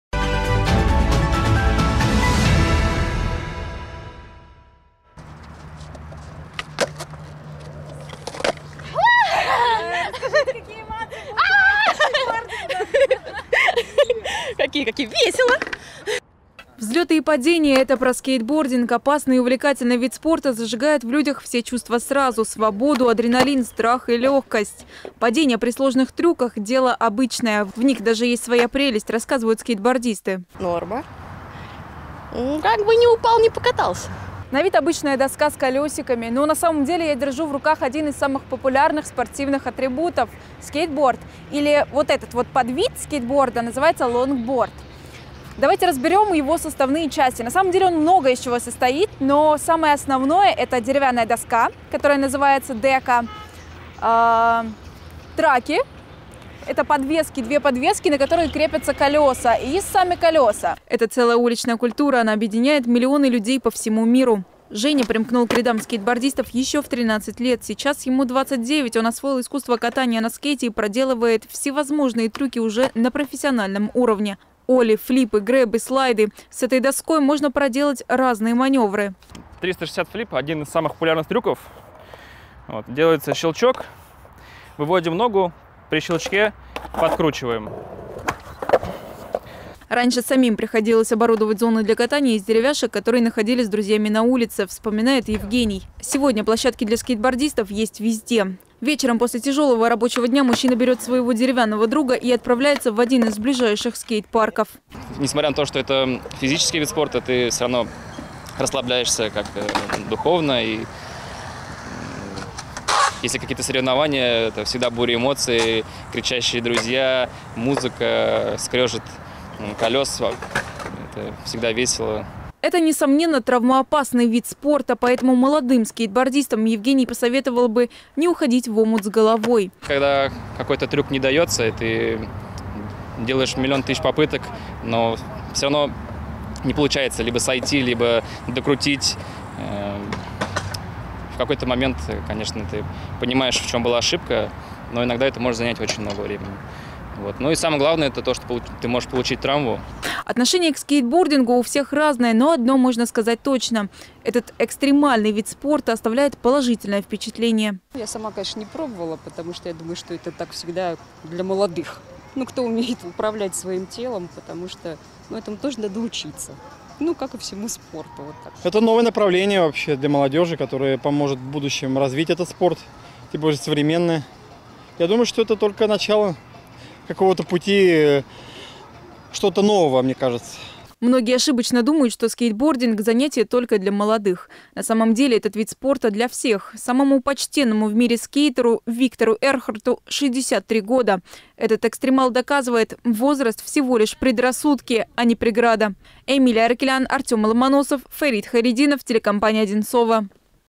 Скейтбординг
Скейтбординг – это целая уличная культура, разнообразные опасные трюки и яркие эмоции. Этот экстремальный вид спорта в последнее время набирает всё большую популярность. Как жители округа относятся к скейтбордингу, чем он так привлекает спортсменов и в чём его сложность?